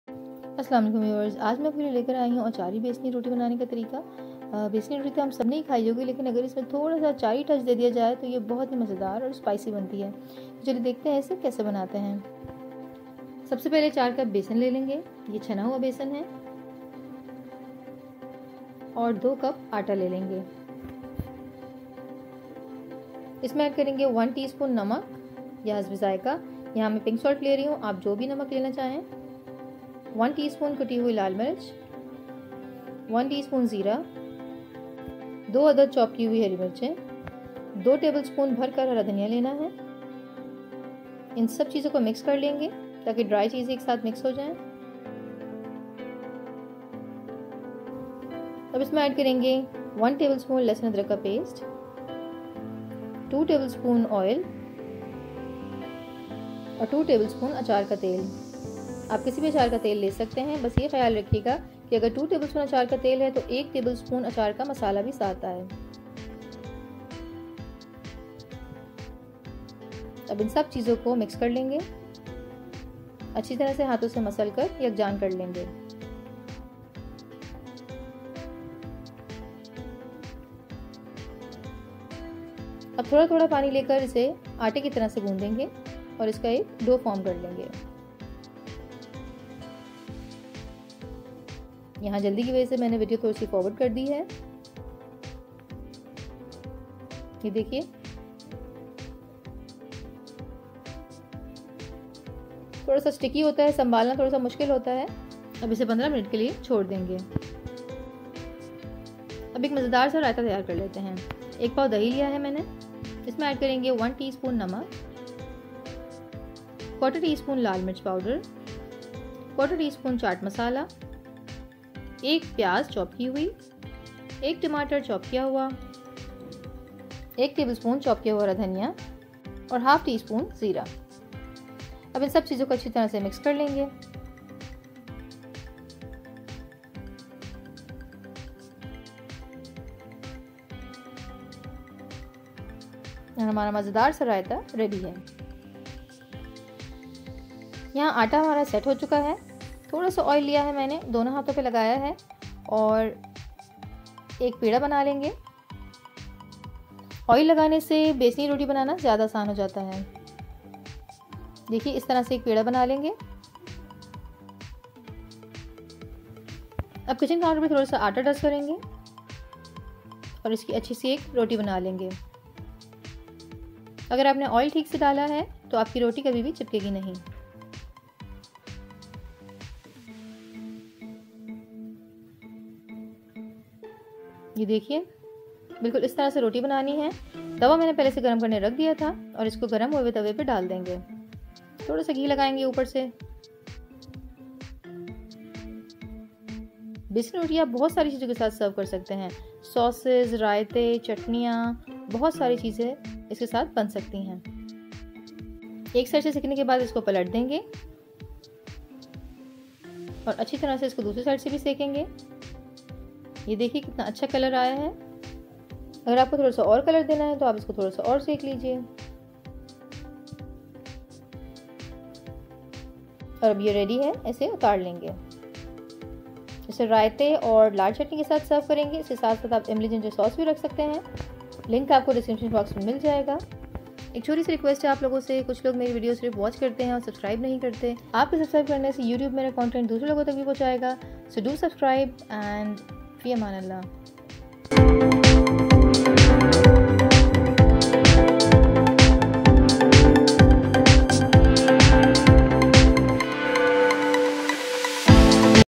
Masalay dar besani roti with magic Raita اچاری بیسنی روٹی
Here I share various kinds of dishes/recipes in my unique style which is very simple to understand with easily available ingredients at home and can be easily cooked so that anyone and everyone can make it.

*Ingredients

4 cup gram flour
2 cup flour
salt to taste
1 tsp red chilli flakes
1 tsp cumin
2 chopped green chillies
2 tbs chopped coriander
2 tbs oil
1 tbs ginger garlic paste
2 tbs pickle oil

*Raita
1 cup yogurt
1 tsp salt
half tsp cumin
quarter tsp chat masala
quarter tsp red chilli powder
1 chopped onion
1 chopped tomato
1 tbs chopped coriander